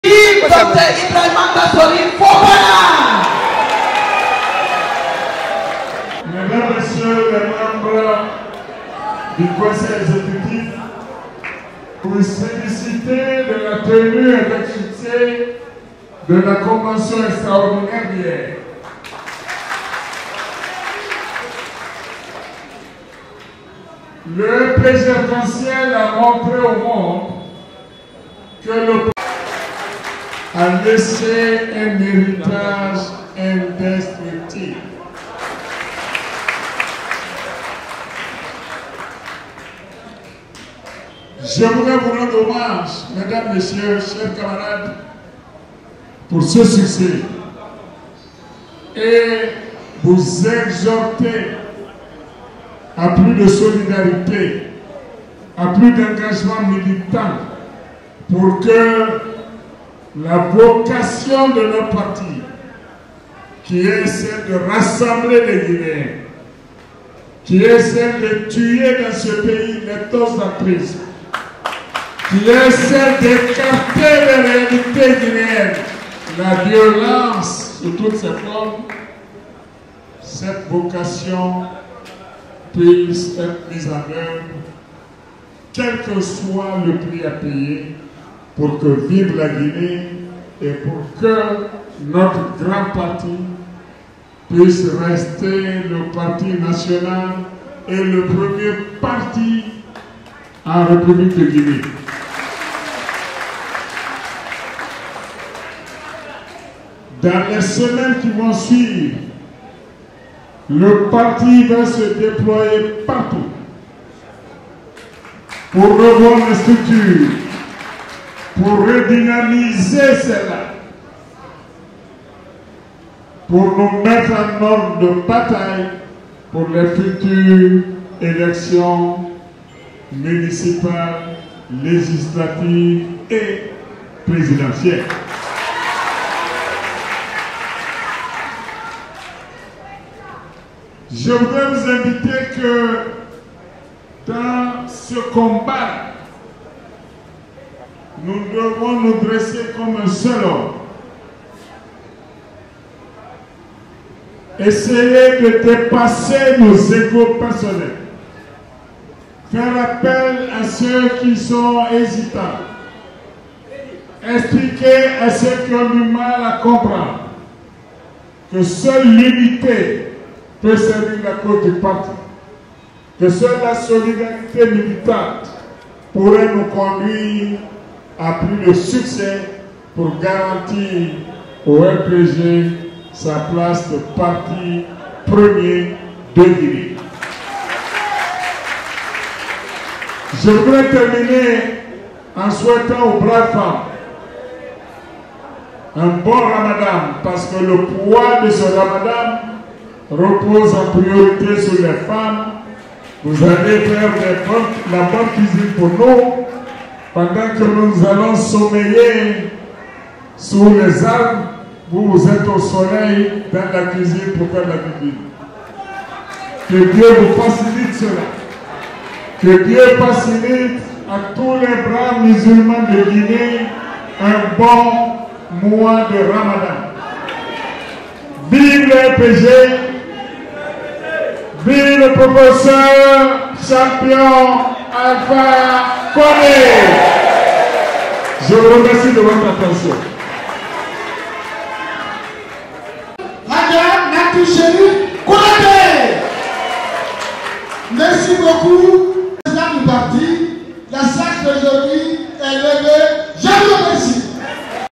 RPG-AEC : Kassory veut « redynamiser » le parti pour « se mettre en ordre de bataille » électorale
Investi officiellement jeudi dernier président du conseil exécutif provisoire du RPG arc-en-ciel, l’ancien Premier ministre, Ibrahima Kassory Fofana, a présidé ce samedi, 2 avril 2022, l’assemblée générale hebdomadaire de ce parti d’Alpha Condé (ancien chef de l’Etat guinéen). Et, au cours de son speech devant les militants et sympathisants de cette formation politique dont il tient désormais les rênes, l’ancien locataire du palais de la colombe a fait part de son intention à redynamiser les structures du RPG arc-en-ciel pour répondre présents à l’ensemble des scrutins qui vont ponctuer et boucler la Transition guinéenne. Et, pour y arriver, il a exhorté à la « solidarité militante » de toutes les forces du parti, rapporte Guineematin.com à travers un de ses journalistes.

Devant les militants mobilisés au siège de son parti à Gbessia, Ibrahima Kassory Fofana a commencé par se réjouir du bon déroulement de la convention nationale extraordinaire qui l’a portée cette semaine à la tête du RPG arc-en-ciel. Il a ensuite laissé entendre que le RPG arc-en-ciel a montré à la face du monde qu’il est encore débout de ses deux pieds. Puis, il a appelé à plus de solidarité pour faire de cette formation politique le premier parti de la Guinée.

« Je tiens à vous féliciter pour la tenue avec succès de la convention extraordinaire d'avant-hier. Le RPG ARC-EN-CIEL a montré au monde que le parti reste debout sur ces deux pieds. Il a démontré à la face du monde que notre champion, le Pr Alpha Condé, a laissé un héritage indestructible. Je voudrais vous rendre hommage mesdames messieurs, chers camarades, pour ce succès et vous exhorter à plus de solidarité, à plus d'engagements militants pour que la vocation de notre parti qui est celle de rassembler les Guinéens, qui est celle de tuer dans ce pays l'ethnocentrisme, qui est celle d'écarter des réalités guinéennes : la violence sur toutes ses formes, cette vocation puisse être mise en œuvre quel que soit le prix à payer pour que vive la Guinée et pour que notre grand parti puisse rester le parti national et le premier parti en république de Guinée », a dit Ibrahima Kassory Fofana.

Par ailleurs, l'ancien Premier ministre d'Alpha Condé a décliné son agent des prochaines semaines pour « mettre en ordre de bataille » électorale le RPG arc-en-ciel.

« Dans les semaines qui vont suivre, le parti va se déployer partout pour revoir les structures, pour redynamiser celles-là, pour nous mettre en ordre de bataille pour les futures élections municipales, législatives et présidentielles. Je voudrais vous indiquer que dans ce come back, nous devons nous dresser comme un seul homme, essayer de dépasser nos égos personnels, faire appel à ceux qui sont hésitants, expliquer à ceux qui ont du mal à comprendre que seul l'unité peut servir la cause du parti, que seul la solidarité militante pourraient nous conduire à plus de succès pour garantir au RPG sa place de parti premier de Guinée », a dit le successeur  d'Alpha Condé à la tête du RPG arc-en-ciel.

A noter que c’est la première assemblée générale hebdomadaire du RPG arc-en-ciel depuis la chute de son champion, Alpha Condé, du pouvoir le 05 septembre dernier.